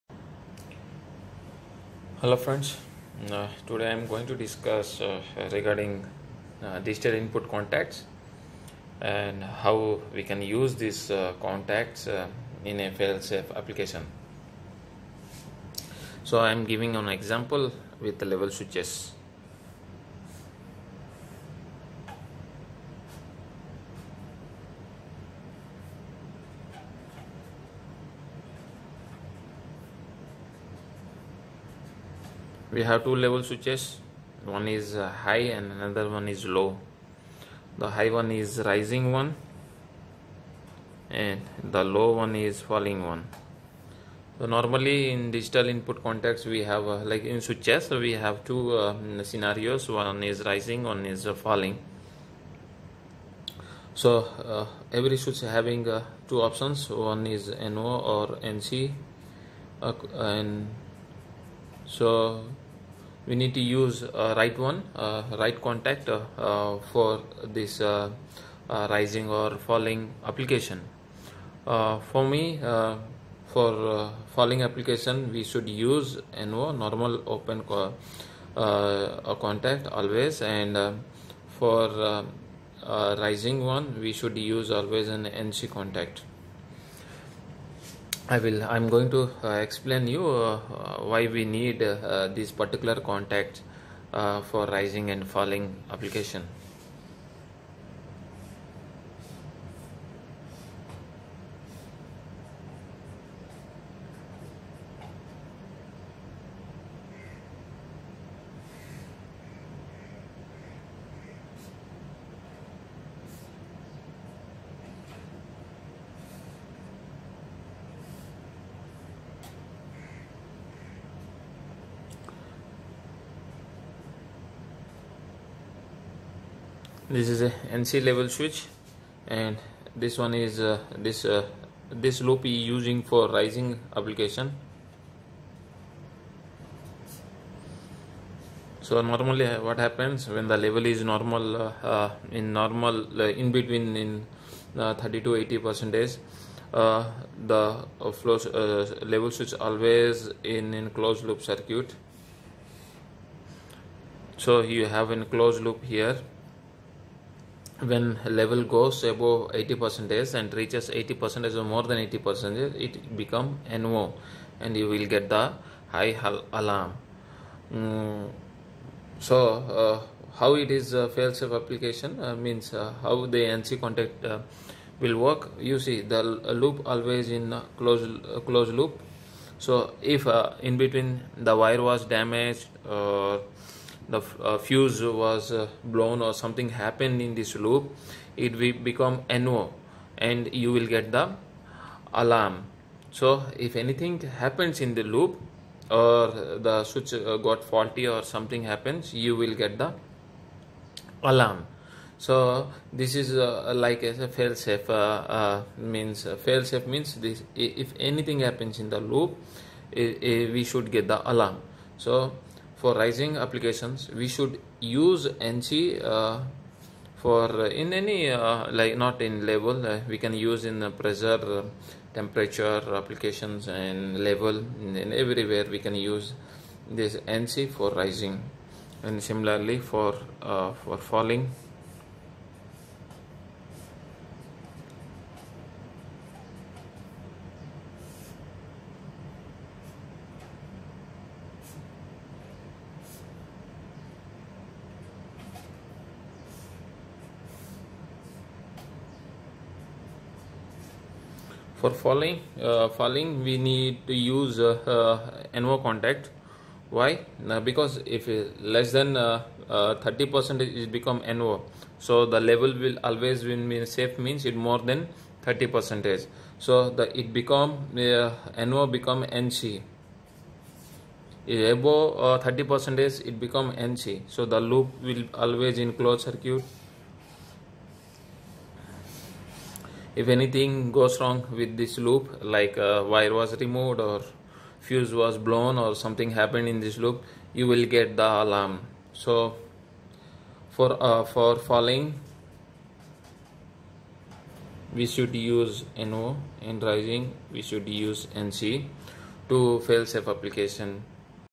Concept of Digital Inputs Contacts (NO-NC)
Concept of Digital Input contacts, how NO contact and NC contact used in fail safe circuits for safe operation, to avoid unwanted events.